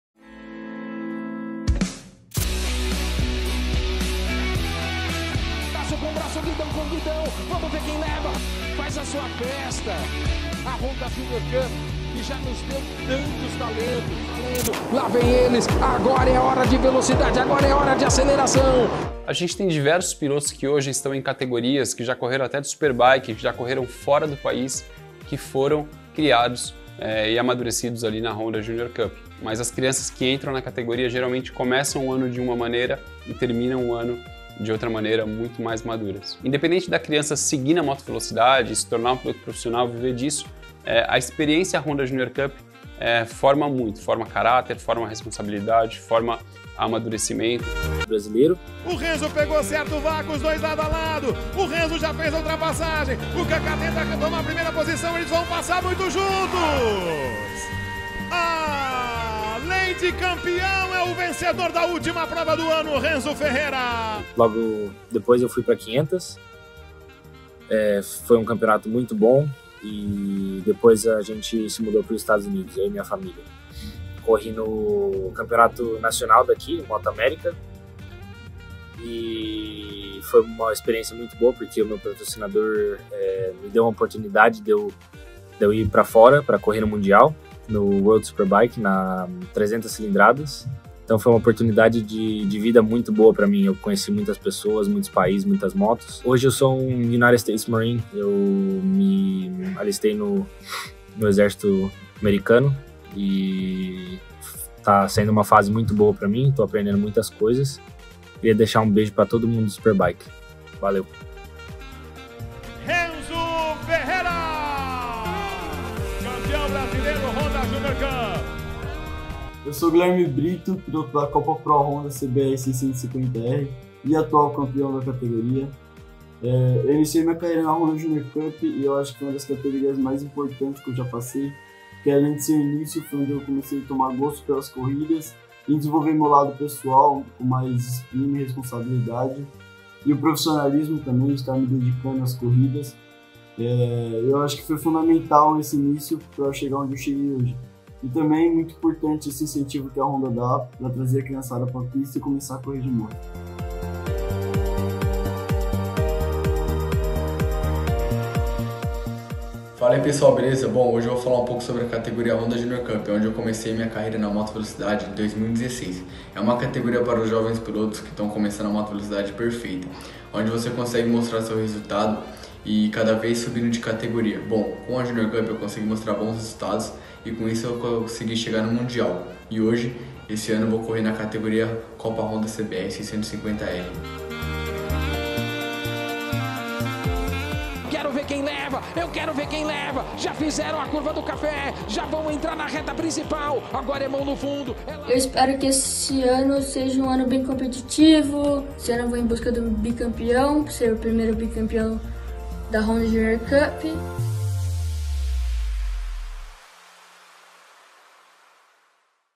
Honda Junior Cup - Episódio 4
Confira todos os detalhes da 4ª Corrida da Honda Junior Cup 2021